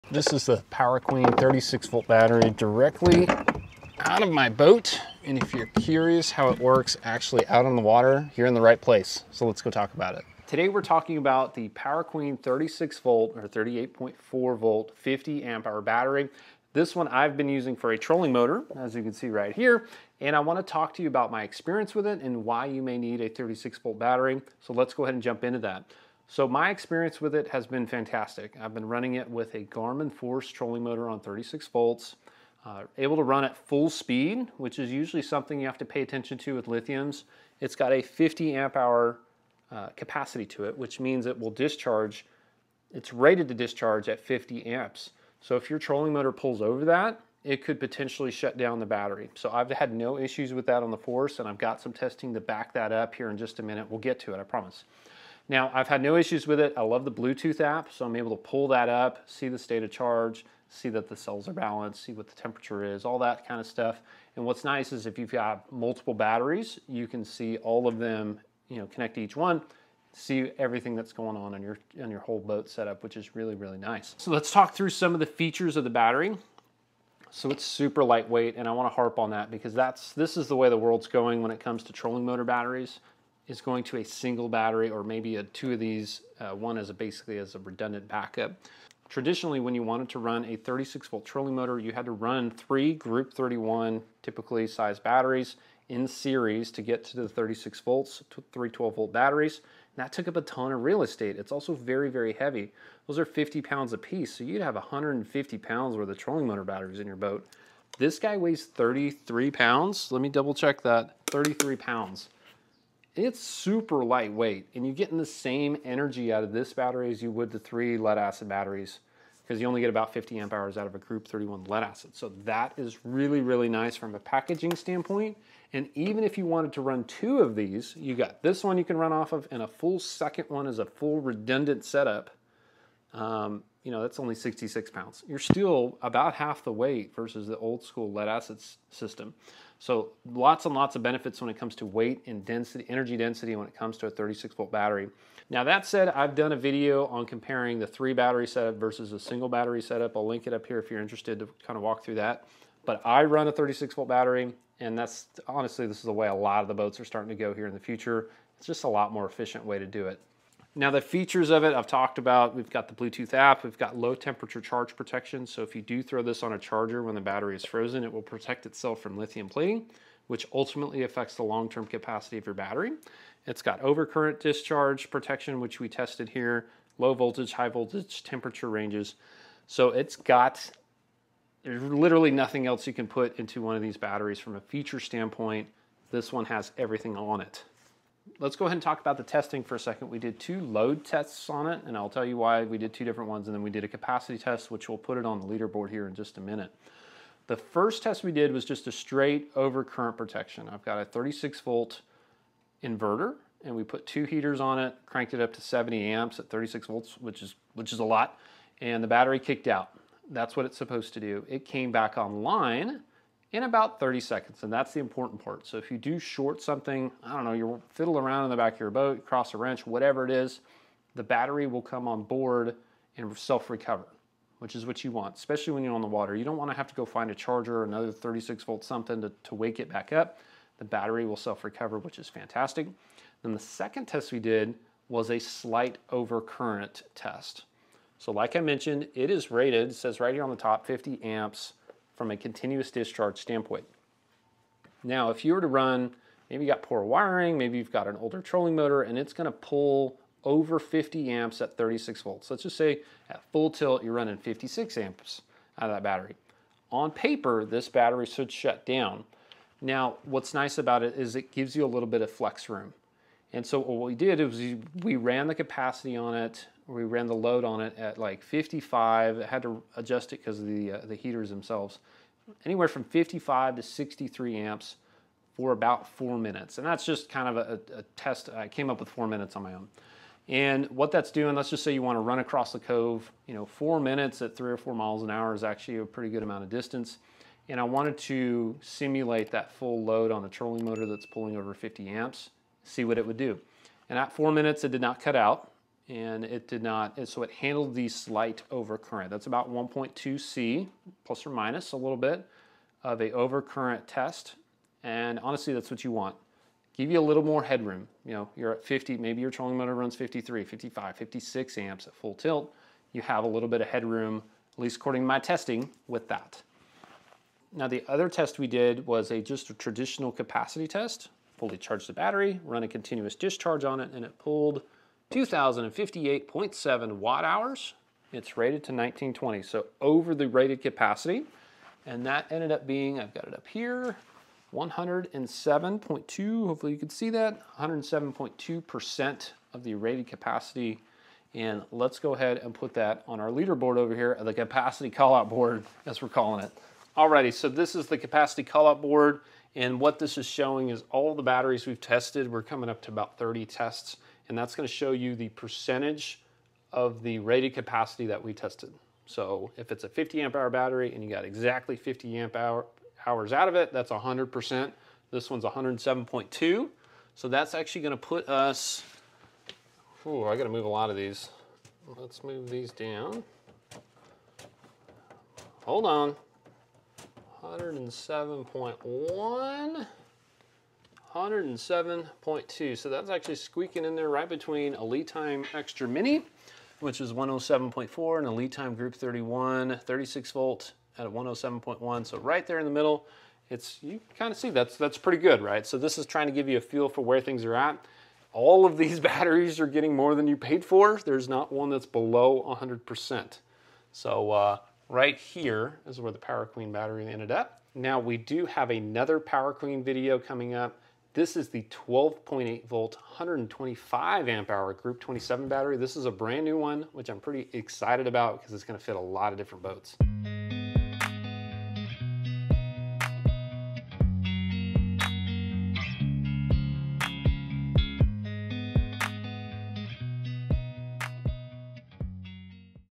Is a Power Queen 36V Worth It Over Lead Acid? Trolling Motor Tested (38.4v)
I put the Power Queen 36V 50Ah lithium battery to the test with my Garmin Force trolling motor to find out how it stacks up against traditional lead acid setups. In this video, I walk through voltage performance, runtime, weight savings, and what kind of real-world gains you can expect when switching to lithium. If you're thinking about upgrading your trolling motor battery system, this hands-on review will help you decide if the Power Queen 38.4V is worth it.

📦 Power Queen 38.4v 50Ah (Amazon)

goodman_fishing operates under Goodman Media LLC.
goodman_fishing is a participant in the Amazon Services LLC Associates and other programs.